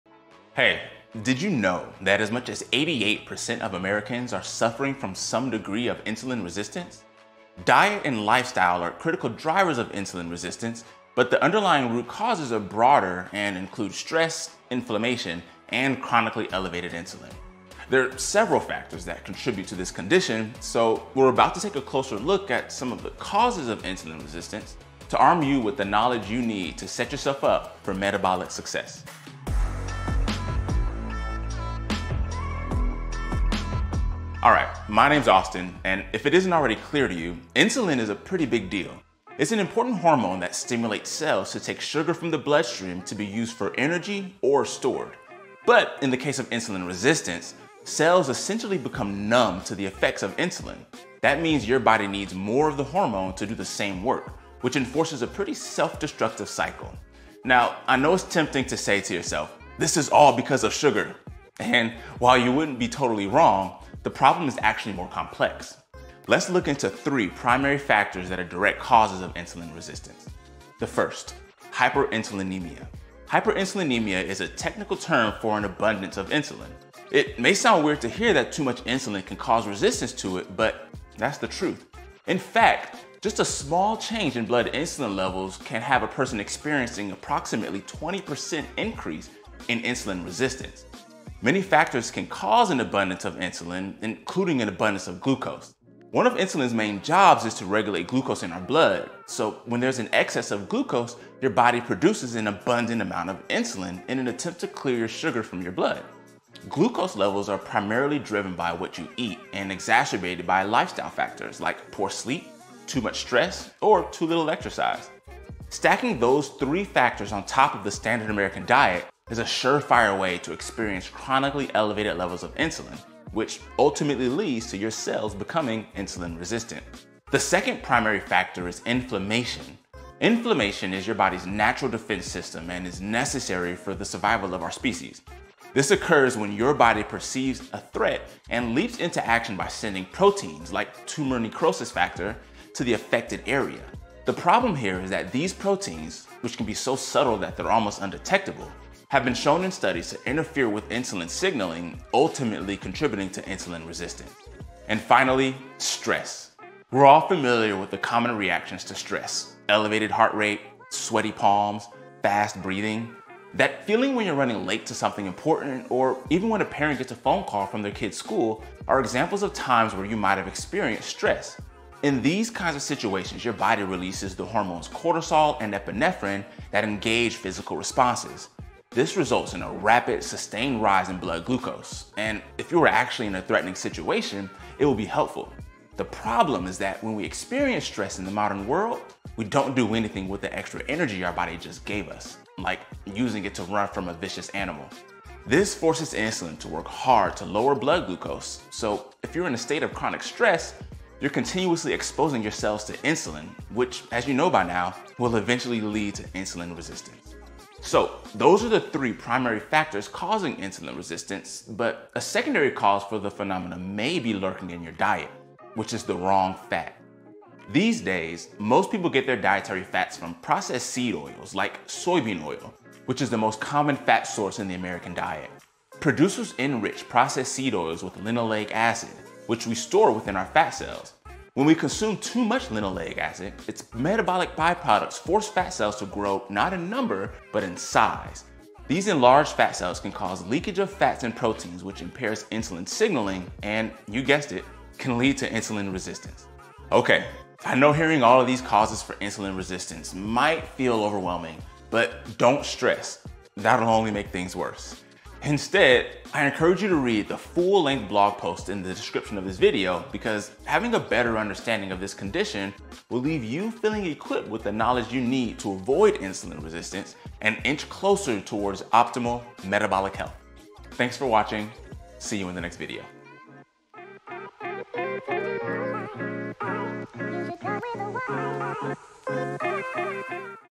4 Surprising Causes of INSULIN RESISTANCE Diet and Lifestyle | Austin McGuffie & Levels
When it comes to our metabolic health, there are several factors that can cause or contribute to insulin resistance. 88% of Americans suffer from some sort of insulin resistance, and the causes extend beyond diet and lifestyle.

Austin McGuffie covers 4 surprising causes of insulin resistance in this video so you can learn how to overcome these and improve your overall health.